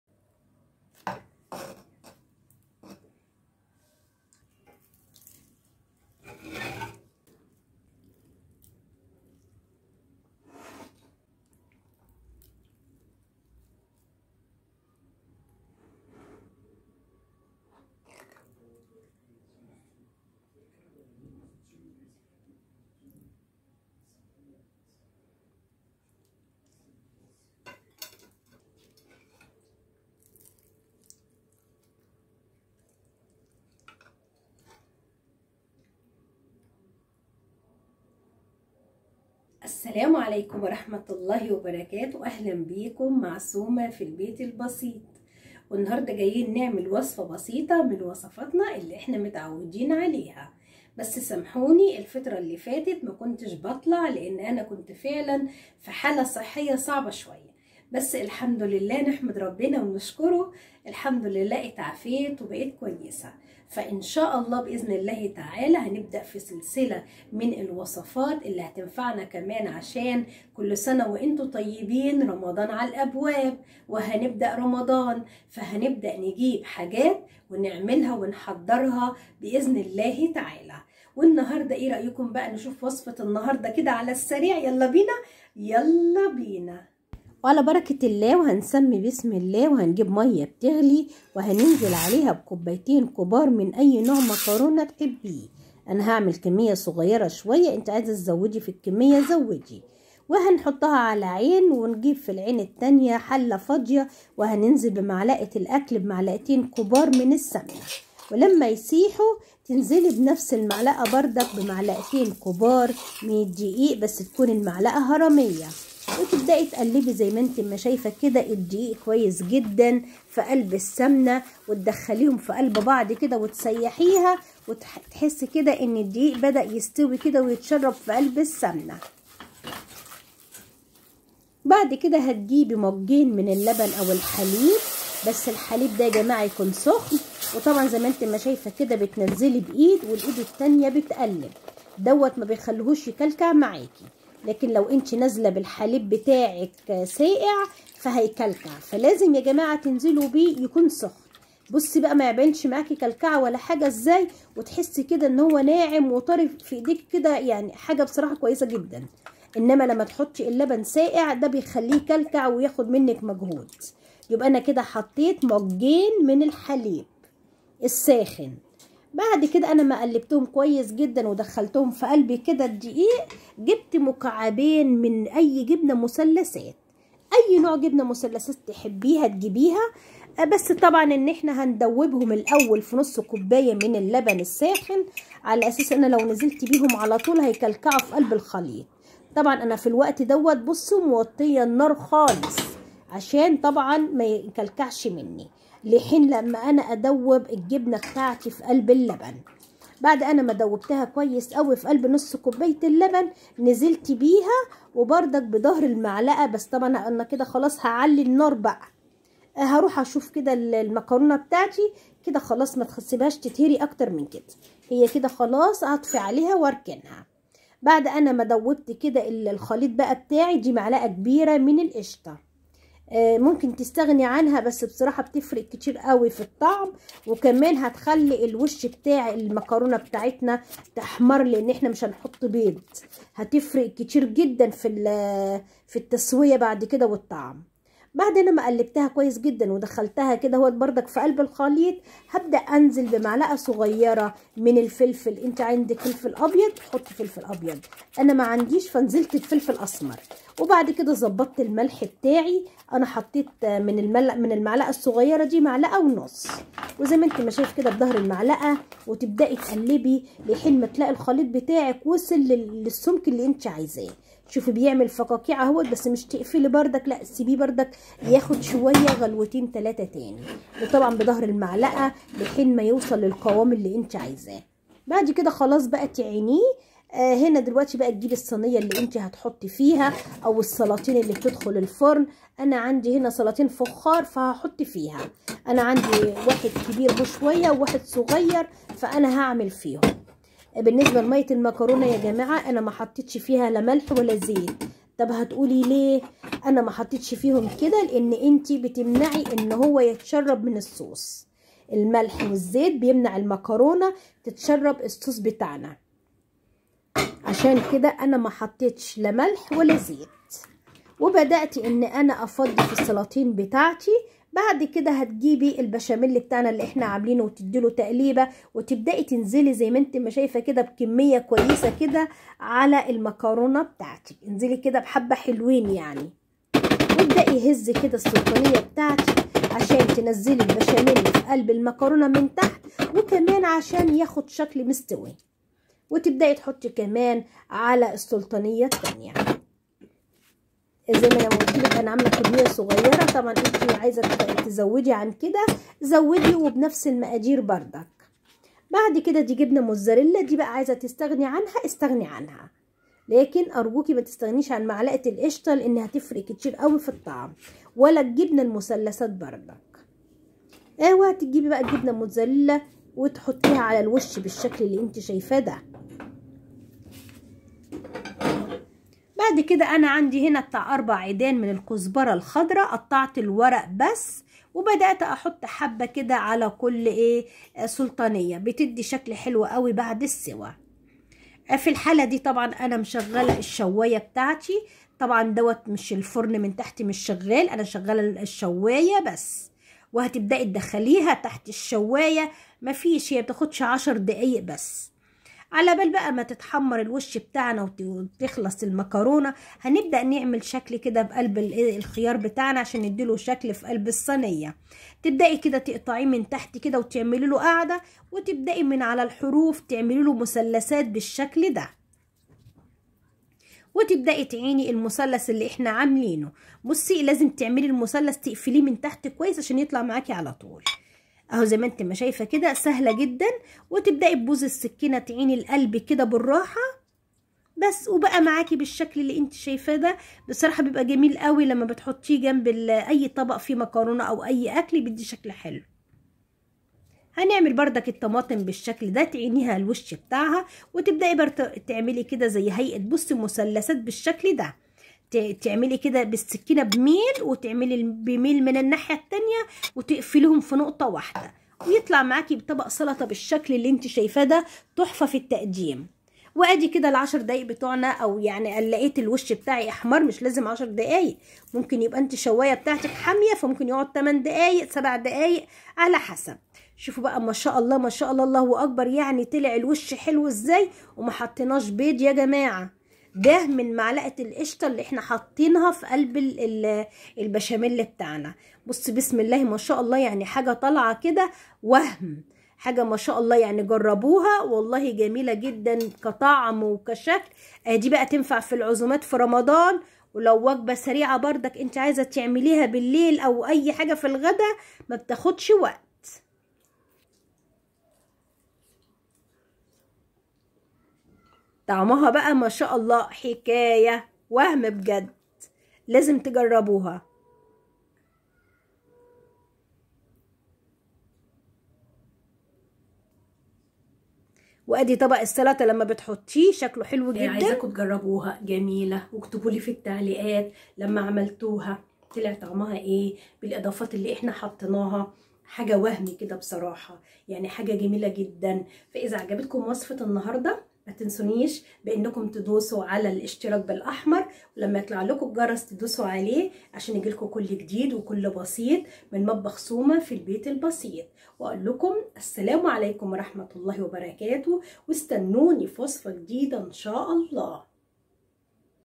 طريقه المكرونه بالوايت صوص بالفخار تحفه بجد وسهله ومن غير تعب فى عزومات رمضان جربيها مش هتندمى
طريقه عمل صوص الابيض للمكرونه بطريقه مختلفه وجميله
طريقه عمل بشمل ناعم وسهل وبسيط وطريقه تخليه قوام ناعم
هخليكى تعملى البشمل بكل سهوله ومن غير مجهود وتعب
وكمان حلوه اوى لعزومات رمضان تشرفك
مع سوما فى البيت البسيط يالا بينا
وبسم الله ونتوكل على الله
المقادير
صلصه بيضاء للمكرونه
معلقتين كبار سمنه
معلقتين كبار دقيق طحين
كوبين من الحليب

قطعتين جبن مثلثات وندوبهم بنصف كوب حليب

معلقه قشطه كبيره

كوبين مكرونه

جبنه مزوريلا لتزين الوجه   وكوزبره خضره الورق فقط